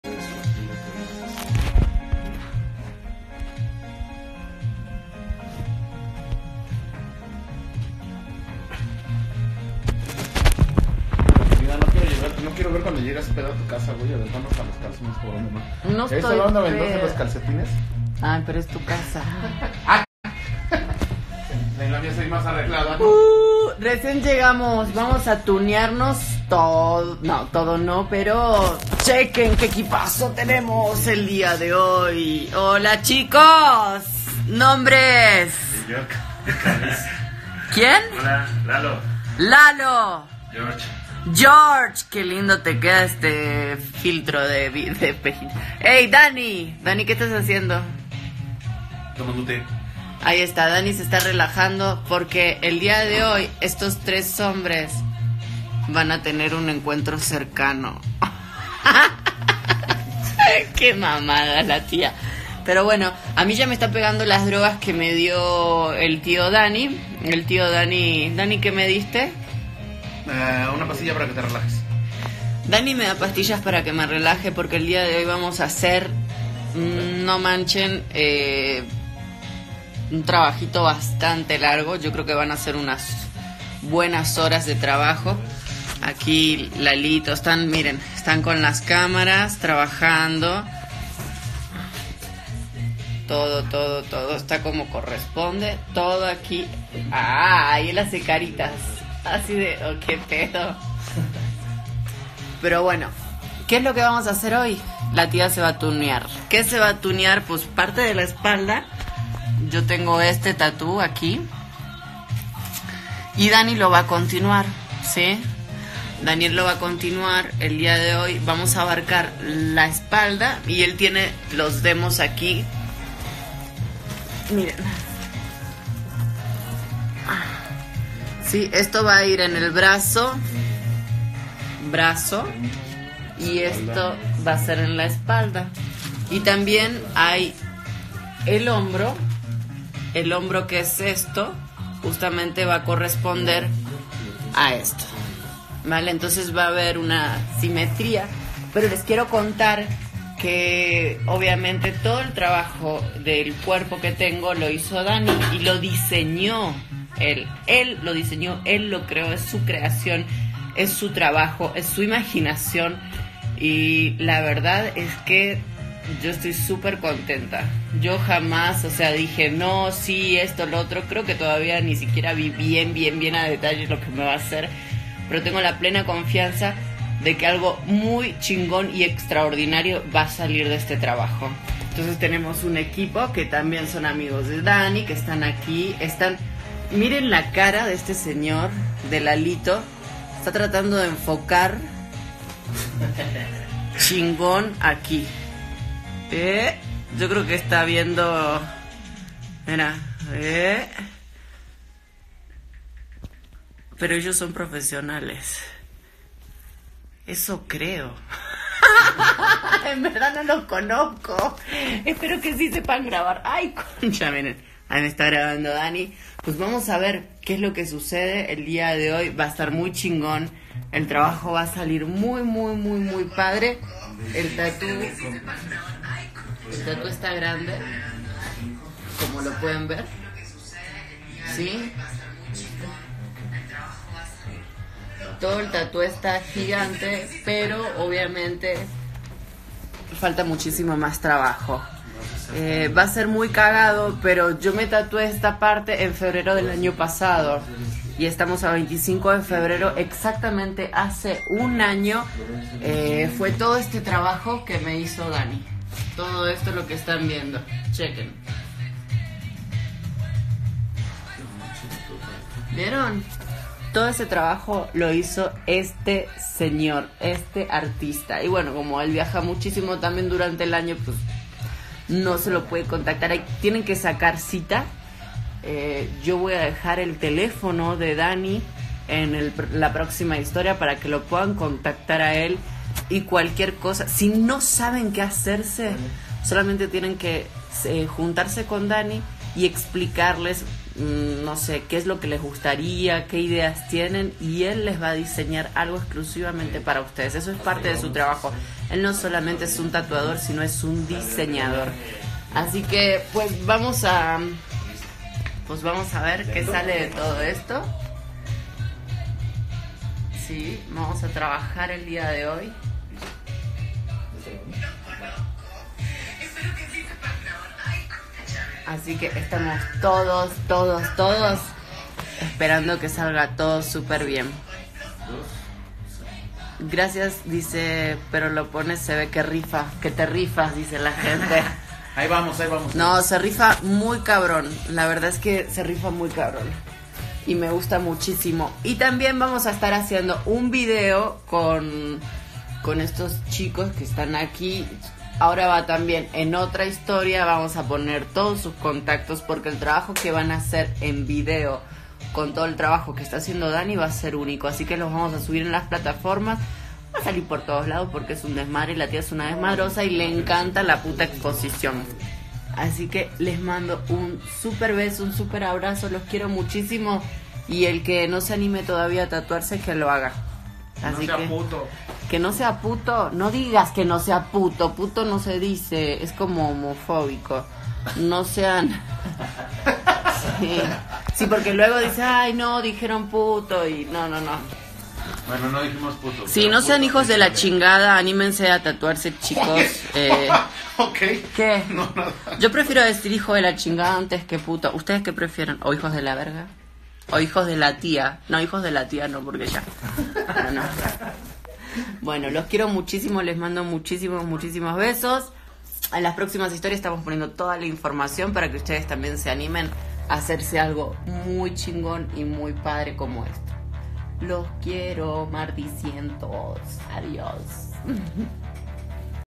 Ingrid Brans - Instagram Live 26 De Febrero De 2019
Grabación integra de la transmisión que hizo desde su cuenta oficial en instagram de la modelo Ingrid Brans alias la reata o ex reata de brozo el dia 26 de febrero de 2019.